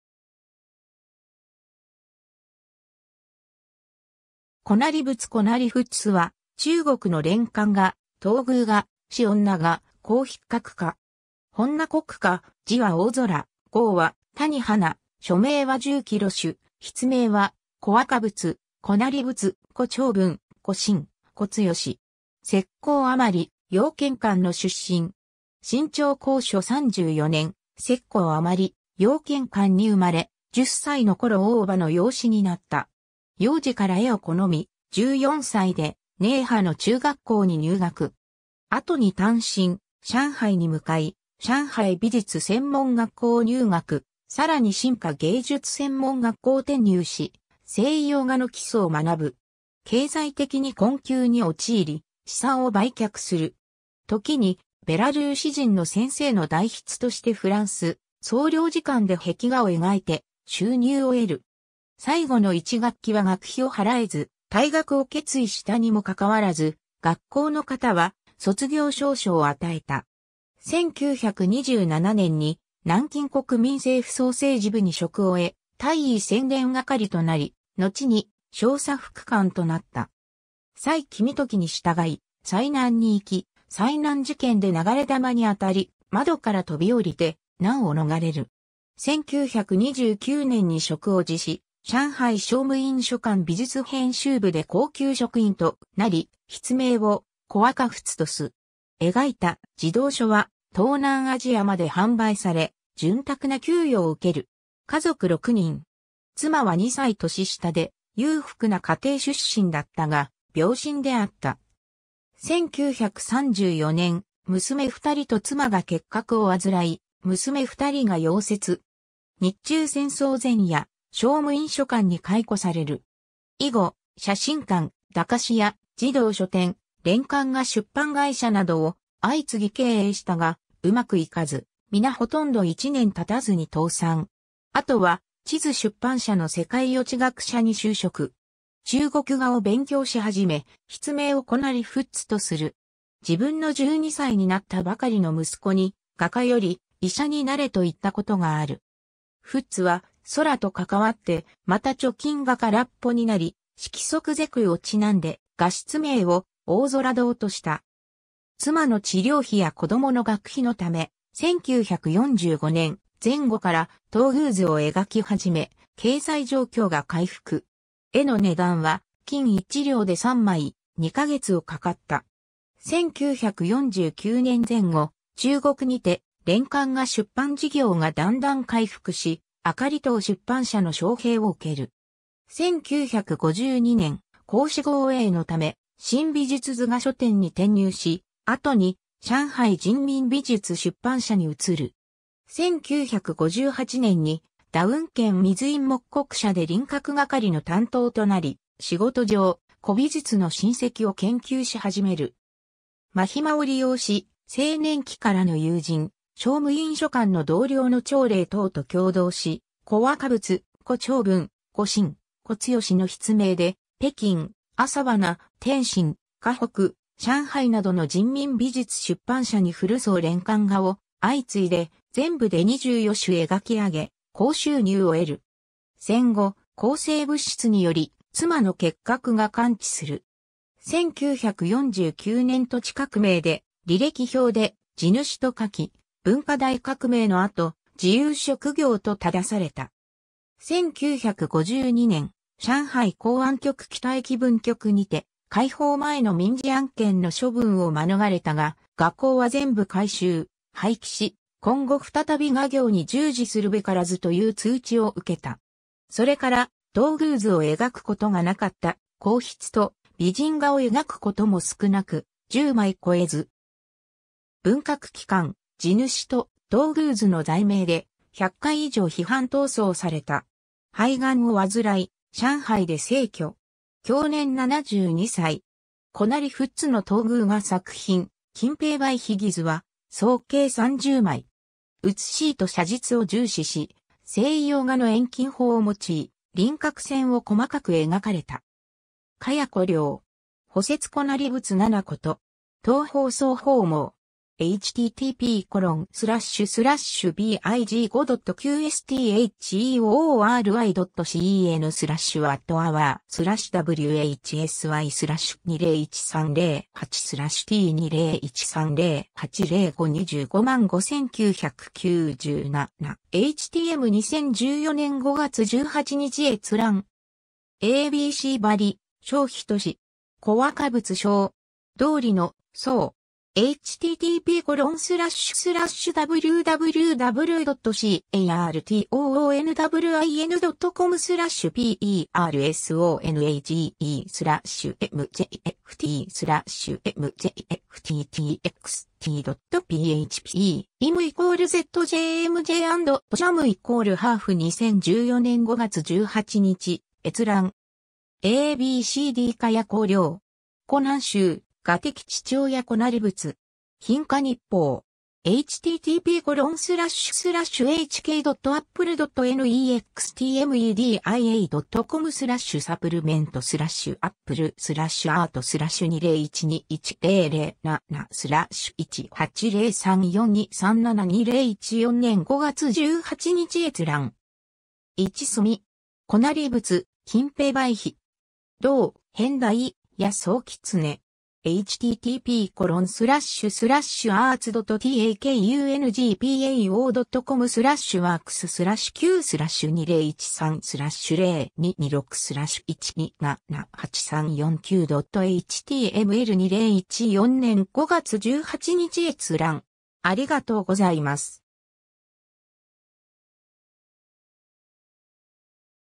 胡也佛
胡也佛 (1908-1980) 胡 也仏（こ やぶつ、フー・イェフォ、繁体字:胡也佛; 簡体字:胡也佛; ピン音:Hú Yěfó; 1908年 - 1980年）は、中国の連環画、春宮画、仕女画、工筆画家。
 本名国華、 字は大空、号は谷華、署名は十卉廬主、 筆名は胡若佛、胡也佛、胡丁文、胡新、胡強 。
浙江余姚県坎墩の出身。
 清朝光緒三十四年（1908年）、浙江余姚県坎墩に生まれ、十歳の頃大叔母の養子になった。
幼時から絵を好み、十四歳で寧波の中学校に入学。
後に単身、上海に向かい、上海美術専門学校入学、さらに新華芸術専門学校転入し、西洋画の基礎を学ぶ。
経済的に困窮に陥り、資産を売却する。
時にベラルーシ人の先生の代筆としてフランス総領事館で壁画を描いて収入を得る。
最後の一学期は学費を払えず、退学を決意したにも関わらず、学校の方は卒業証書を与えた。
1927年に南京国民政府総政治部に職を得、大尉宣伝係となり、のちに少佐副官となった。
蔡公時に従い済南に行き、済南事件（五・三惨案）で流れ弾に当たり、窓から飛び降りて難を逃れる。
 1929年に職を辞し、上海商務印書館美術編集部で高級職員となり、筆名を胡若仏とす。
描いた児童書は東南アジアまで販売され、潤沢な給与を受ける。
家族六人。
妻は二歳年下で、裕福な家庭出身だったが、病身であった。
1934年、娘二人と妻が結核を患い、娘二人が夭折。
日中戦争前夜、商務印書館に解雇される。
以後、写真館、駄菓子屋、児童書店、連環画出版会社などを相次ぎ経営したが、うまく行かず、皆ほとんど一年経たずに倒産。
後は地図出版社の世界輿地学社に就職。
中国画を勉強し始め、筆名を胡也仏とする。
自分の十二歳になったばかりの息子に、画家より医者になれと言ったことがある。
「仏」は「空」と関わって、また貯金が空っぽになり、「色即是空」を因んで、画室名を「大空堂」とした。
妻の治療費や子供の学費のため、1945年前後から春宮図を描き始め、経済状況が回復。
絵の値段は金一両で3枚、二ヶ月をかかった。
1949年前後、中国にて連環画出版事業がだんだん回復し、燈塔出版社の招聘を受ける。
1952年、公私合営のため、新美術図画書店に転入し、後に上海人民美術出版社に移る。
1958年に「朶雲軒」水印木刻社で輪郭係の担当となり、仕事上、古美術の真跡を研究し始める。
間暇を利用し、青年期からの友人、商務印書館の同僚の張令濤と協同し、「胡若佛」、「胡丁文」、「胡新」、「胡強」の筆名で、北京、朝花、天津、河北、上海などの人民美術出版社に古装連環画を、相次いで、全部で二十余種描きあげ、高収入を得る。
戦後、抗生物質により、妻の結核が完治する。
 1949年土地革命で、履歴表で「地主」と書き、文化大革命の後「自由職業」と正された。
1952年、上海公安局北駅分局にて「解放前の民事案件の処分を免れたが、画稿は全部回収、廃棄し、今後再び画業に従事するべからず」という通知を受けた。
それから春宮図を描くことがなかった、工筆と美人画を描くことも少なく、十枚超えず。
文革期間、地主と春宮図の罪名で、百回以上批判闘争された。
 肺癌を患い、上海で逝去。
享年七十二歳 。
 胡也仏の春宮画作品『金瓶梅秘戯図』は総計三十枚。
写意と写実を重視し、西洋画の遠近法を用い、輪郭線を細かく描かれた 。
^ 茅子良 (2013年8月15日). “補説胡也佛七事” (中国語). 東方早報網. 2014年5月18日閲覧。
^ a b c 張頌仁. “胡若佛小传” (中国語). 卡通之窗. 2014年5月18日閲覧。
^ a b c d 茅子良 (2012年10月17日). “胡南洲：我的父親胡也佛” (中国語). 「蘋果日報」. 2014年5月18日閲覧。
^ 一墨 (2013年2月26日). “胡也佛「金瓶梅秘戏图」艺术鉴赏” (中国語). 读书频道图片库-大视野-搜狐.